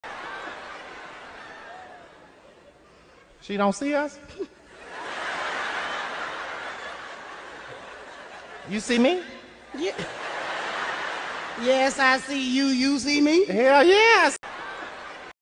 Needy dogs be like…
When you can’t even have a conversation on the phone with your friends without your dogs feeling like you abandoned them!